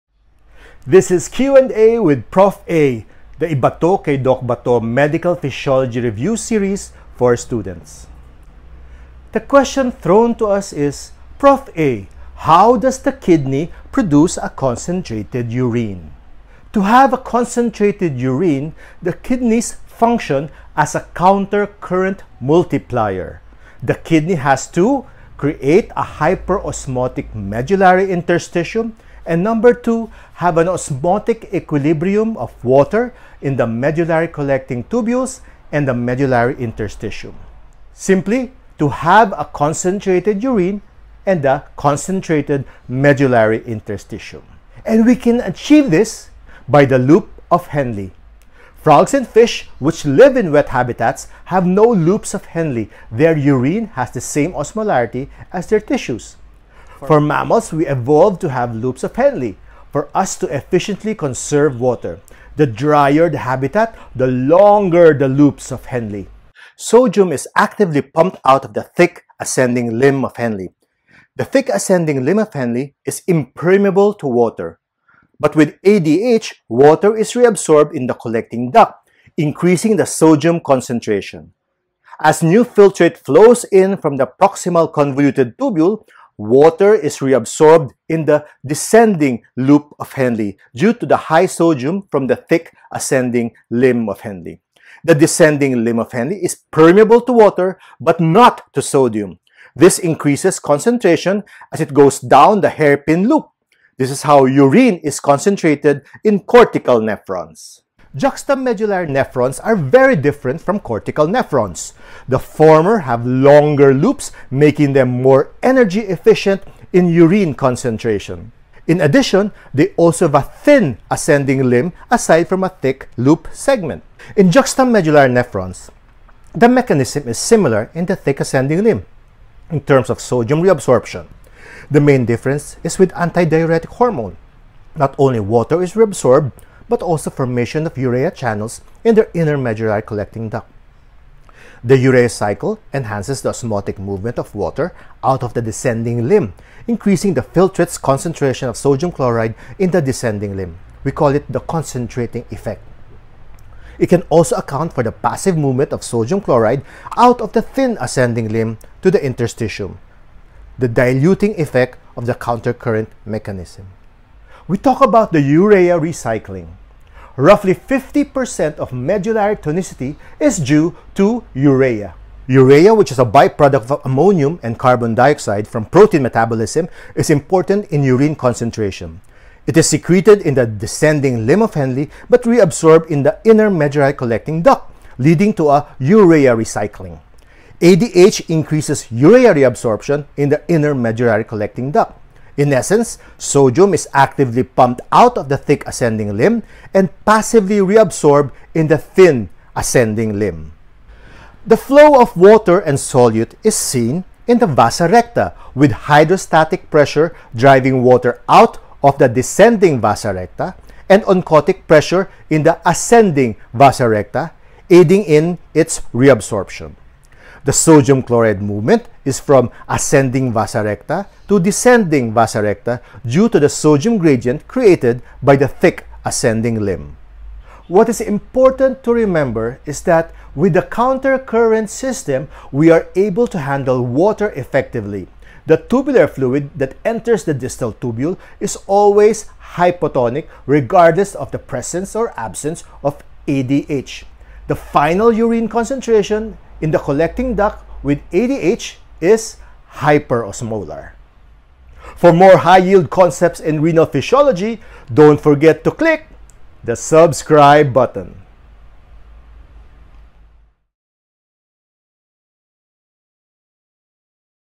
High-Yield Renal Physiology: Countercurrent Mechanism
For medical students and other health practitioners interested in understanding how the kidney works. Great for bedside rounds and even exams.